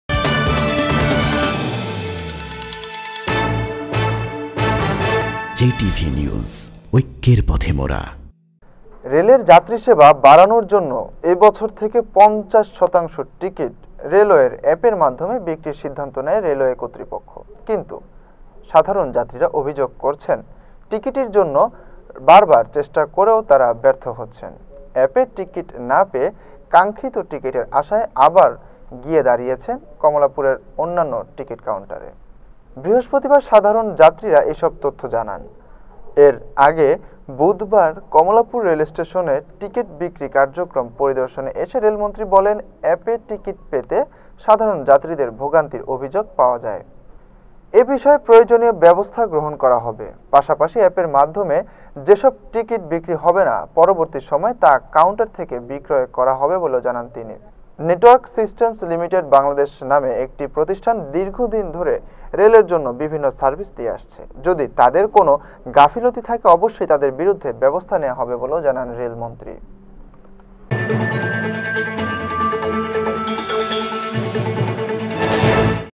JTV | অ্যাপে মিলছে না টিকিট, ভিড় বাড়ছে কাউন্টারে | JTV News
রেলের যাত্রীসেবা বাড়ানোর জন্য এ বছর থেকে ৫০ শতাংশ টিকিট রেলওয়ের অ্যাপের মাধ্যমে বিক্রির সিদ্ধান্ত নেয় রেলওয়ে কর্তৃপক্ষ। কিন্তু সাধারণ যাত্রীরা অভিযোগ করছেন, টিকিটের জন্য বারবার চেষ্টা করেও তারা ব্যর্থ হচ্ছেন। অ্যাপে টিকিট না পেয়ে কাঙ্ক্ষিত টিকিটের আশায় আবার গিয়ে দাঁড়িয়েছেন কমলাপুরসহ অন্যান্য টিকিট কাউন্টারে। বৃহস্পতিবার সাধারণ যাত্রীরা এসব তথ্য জানায়। এর আগে বুধবার কমলাপুর স্টেশনে টিকিট বিক্রি কার্যক্রম পরিদর্শনে এসে রেলমন্ত্রী বলেন, ‘অ্যাপে টিকিট পেতে সাধারণ যাত্রীদের ভোগান্তির অভিযোগ পাওয়া গেছে। এ বিষয়ে প্রয়োজনীয় ব্যবস্থা গ্রহণ করা হবে। পাশাপাশি অ্যাপের মাধ্যমে যেসব টিকিট বিক্রি হবে না, পরবর্তী সময়ে তা কাউন্টার থেকে বিক্রয় করা হবে বলে জানান তিনি। নেটওয়ার্ক সিস্টেমস লিমিটেড বাংলাদেশ নামে একটি প্রতিষ্ঠান দীর্ঘদিন ধরে রেলের জন্য বিভিন্ন সার্ভিস দিয়ে আসছে। যদি তাদের কোনও গাফিলতি থাকে অবশ্যই তাদের বিরুদ্ধে ব্যবস্থা নেওয়া হবে বলেও জানান রেলমন্ত্রী।